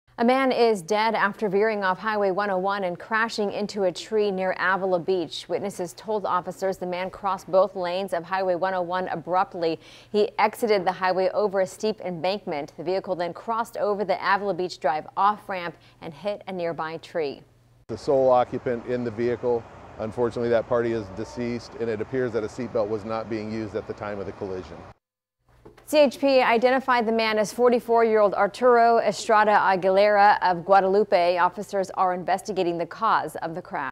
CHP identifies man killed in crash near Avila Beach
The California Highway Patrol has identified the man killed in a vehicle crash Tuesday morning near Avila Beach as Arturo Estrada-Aguilera, 44, of Guadalupe.